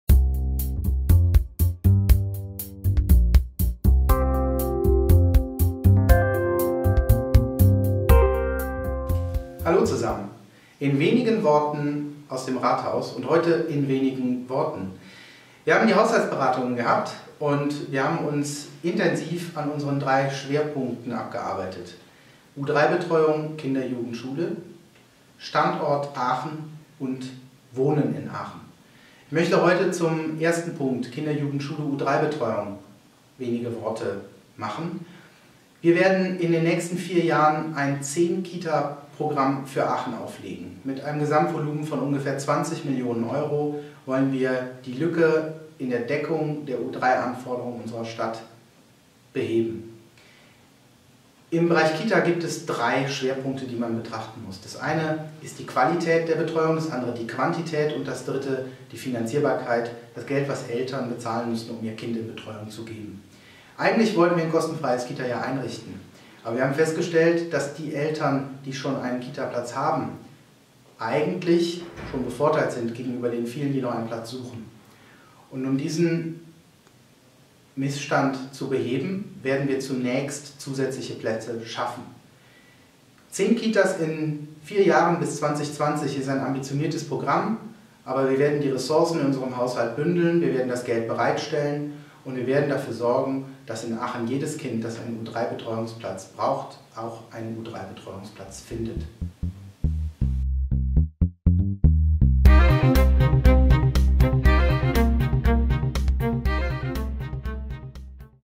inwenigenworten... Michael Servos zu den Haushaltsberatungen - Thema Kinder/Jugend/Schule/U3
In seinem Vlog erklärt Michael Servos, der Fraktionsvorsitzende der SPD Ratsfraktion, die Ergebnisse der Haushaltsberatung der Ratsmehrheit im Aachener Stadtrat in drei kurzen Teilen.